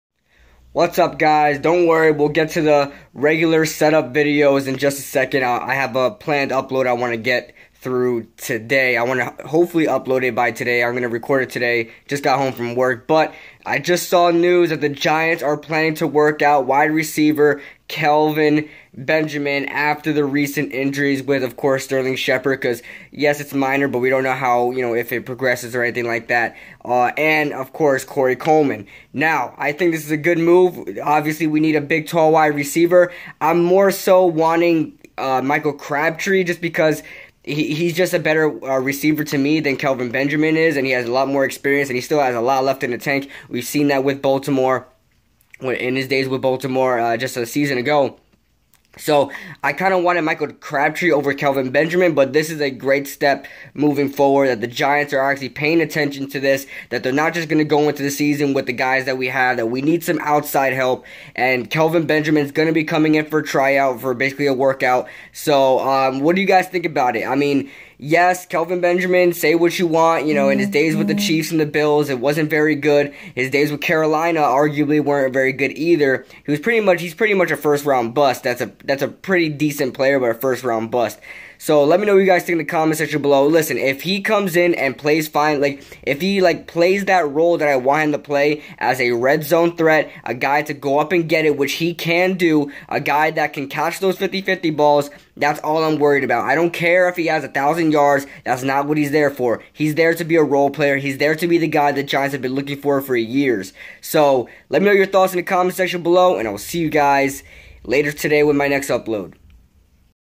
UPDATE: Giants to work out WR Kelvin Benjamin on Saturday!
Giants are bringing in WR Kelvin Benjamin for a work out. Benjamin has been talked about as a potential signing for Big Blue for months. Will it happen ?